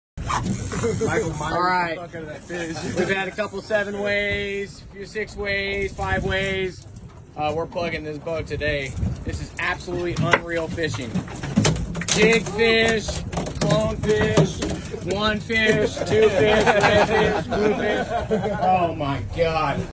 Albacore Tuna Fishing
Some Albacore Tuna action on Pacific Angler Sportfishing run up north in Ft. Bragg, CA.